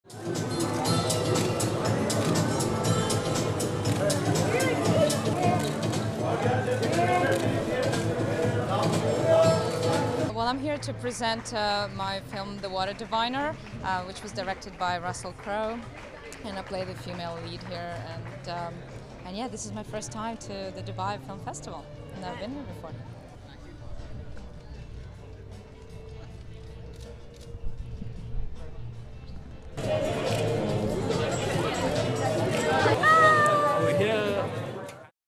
EVENT CAPSULE CLEAN - 'The Water Diviner' Red Carpet - 11th Annual Dubai International Film Festival
EVENT CAPSULE CLEAN - 'The Water Diviner' Red Carpet - 11th Annual Dubai International Film Festival at Madinat Jumeirah on December 11, 2014 in Dubai, United Arab Emirates.

Featuring:
    Olga Kurylenko